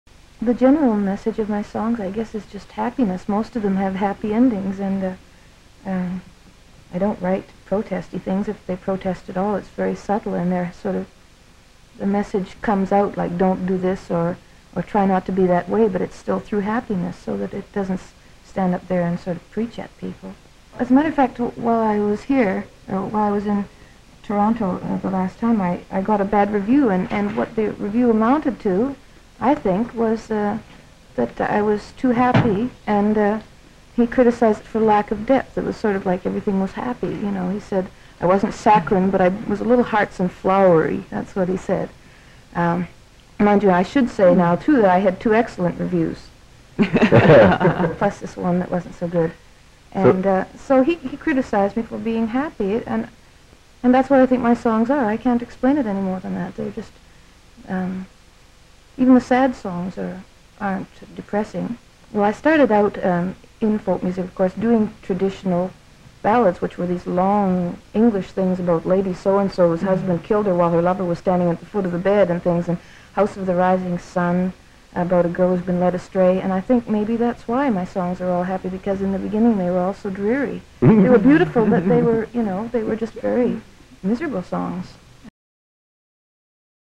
Joni Mitchell told music 'lacks depth' (1967)
Joni Mitchell on being criticized for a "lack of depth" in her music during this 1967 CBC Interview.

Joni is one of our Top 30 women who have changed music -- check out the full list, and let us know your picks!